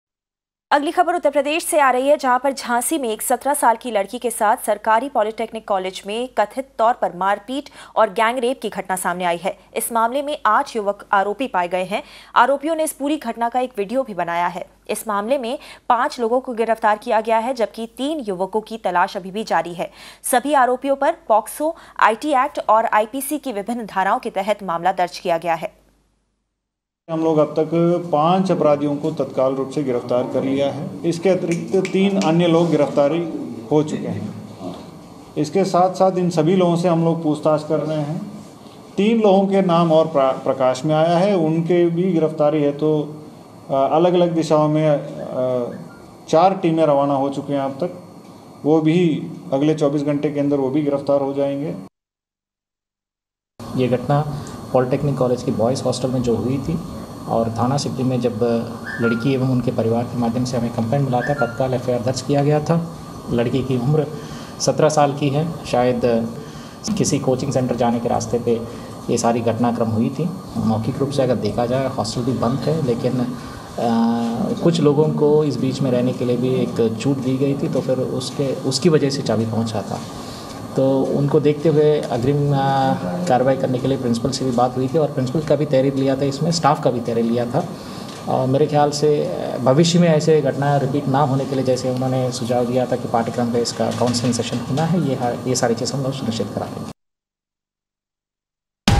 UP Shocker: Teenage girl allegedly gangraped in a Govt Polytechnic College
UP: A horrific incident has come to light from Uttar Pradesh's Jhansi. A 17 year old girl has been allegedly gangraped at a Government Polytechnic College. The teen girl has been thrashed, raped and filmed. The boys allegedly made a video of the horrific incident. 8 boys have assaulted the girl in the college hostel. 5 Boys have arrested whereas 3 are still absconding.

Dinesh Kumar SSP, Jhansi has said "We have arrested the 5 accused in the case and the search is on for other 3 people whose names have come to light. We are questioning the accused in the case."